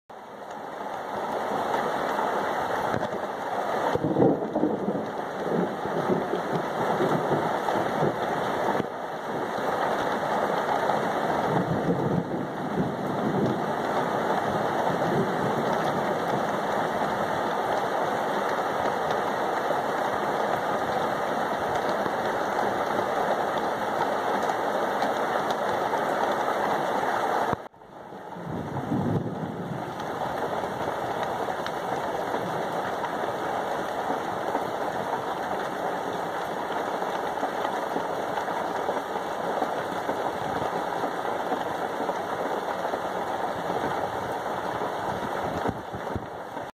smokin in the rain
Can't let a little thunder n lightning stop me